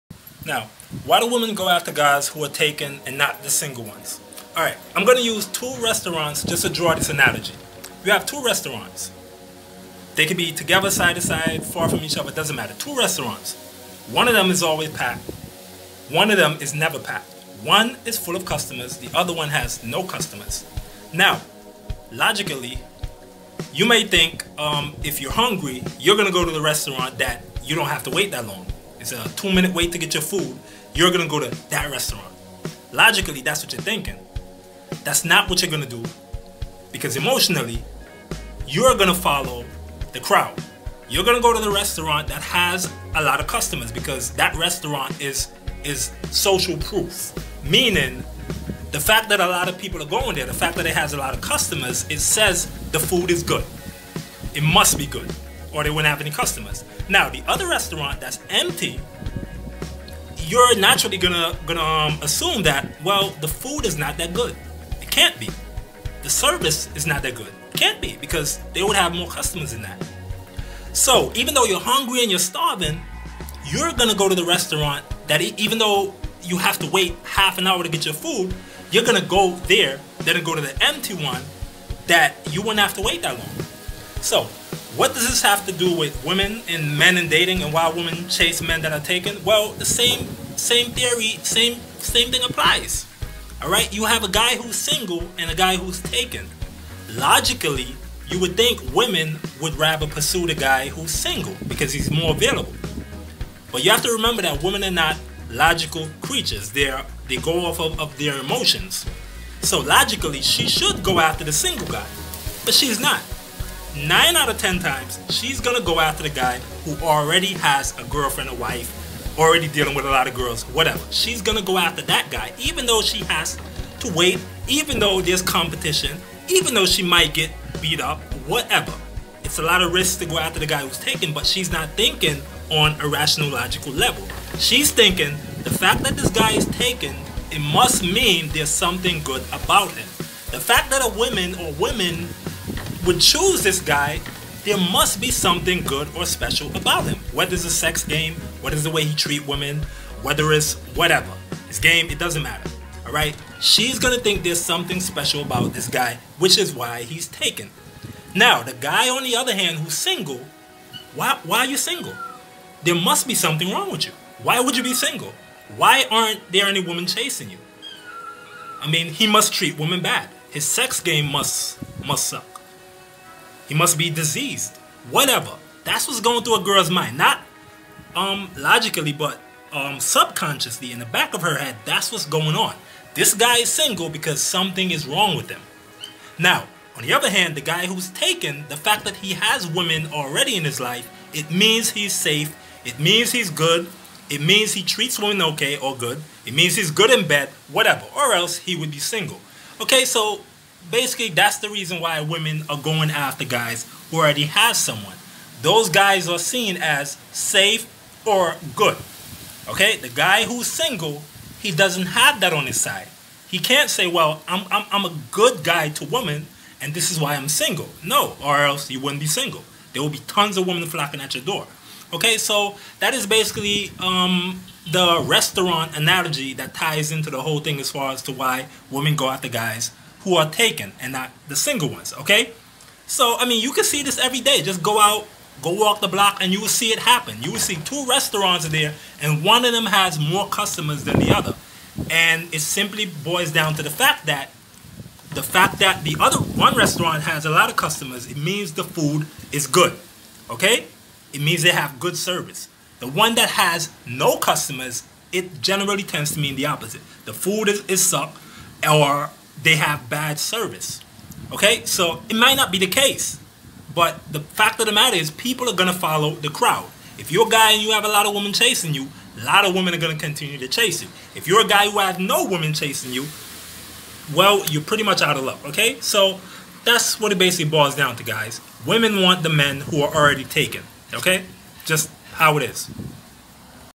Why Women Go After Men Who Are Taken👫 RedPill💊
Why do women go after men who are taken?

It is all psychological.

Women want what orher women have.

In this video, I talked about social proof and being preselected, when it comes to meeting women in dating and mating.

Also check out a more recent video on the topic of women going after men who are taken. Also, why you should never tell women that you're single!